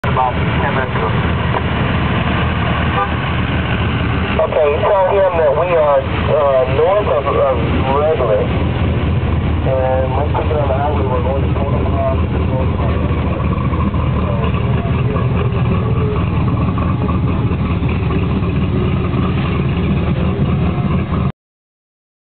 Vegas to Reno
Trophy Truck #31 put the truck on the trailer at race mile 333.
more info log onto... Andy McMillin.com, skgauto.com